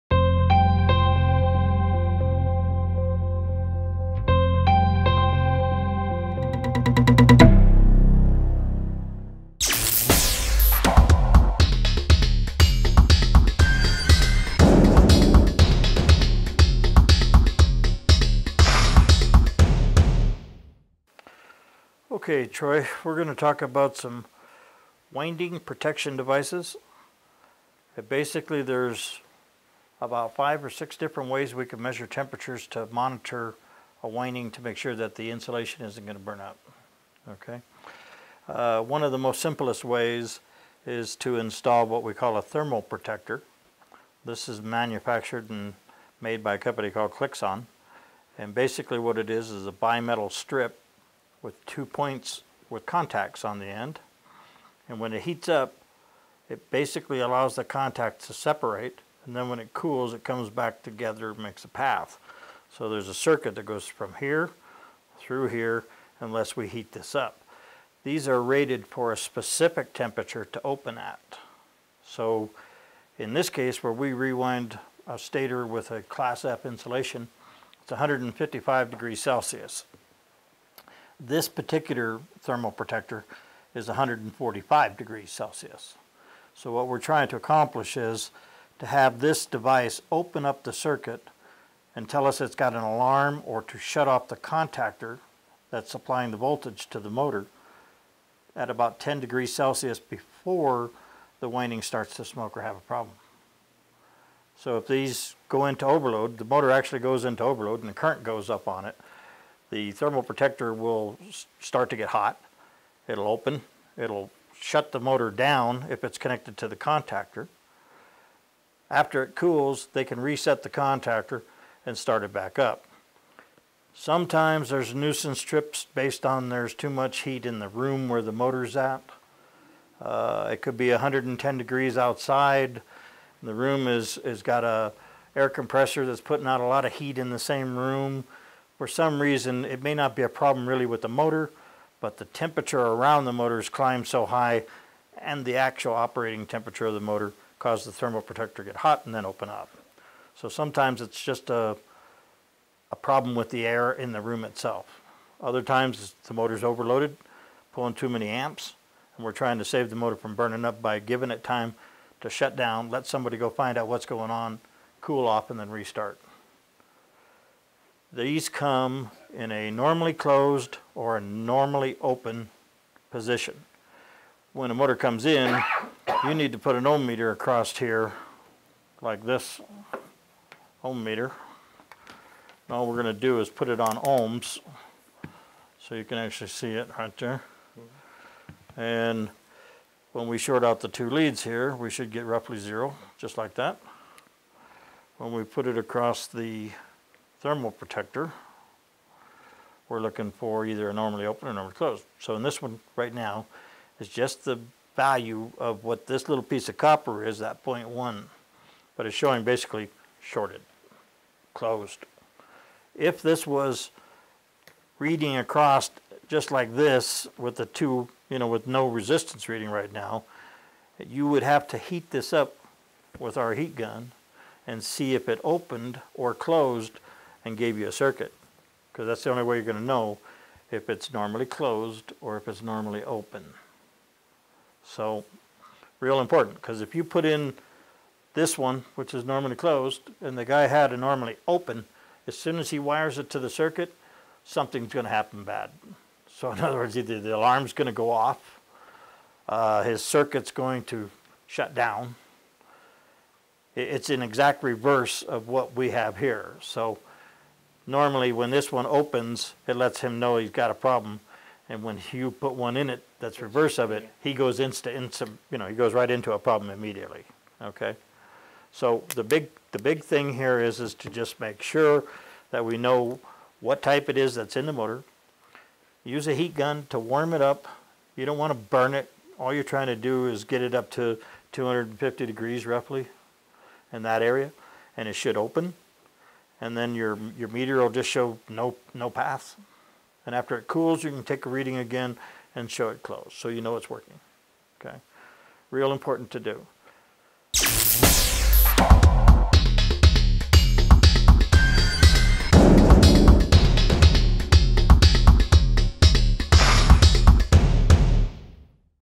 Temperature Devices - Part 1 - Klixons
Part 1 discusses the different thermal protection devices embedded into windings. You'll learn the types, testing, and an understanding of temp ratings.